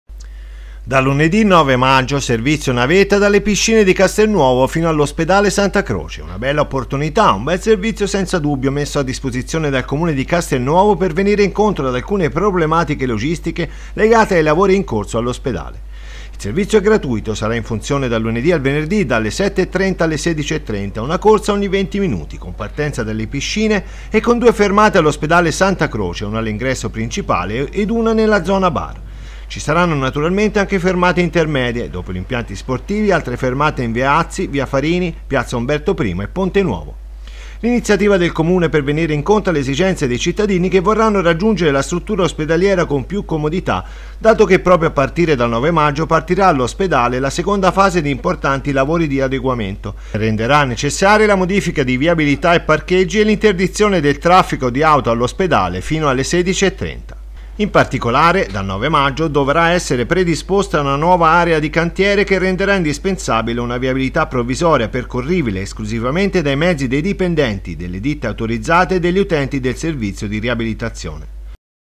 Meno posti auto all'ospedale di Castelnuovo per lavori; il Comune attiva un servizio navetta
Le corse gratuite scatteranno lunedì 9 maggio: partiranno ogni 20 minuti, dal lunedì al venerdì, nella fascia oraria 7:30-16:30.

NoiTv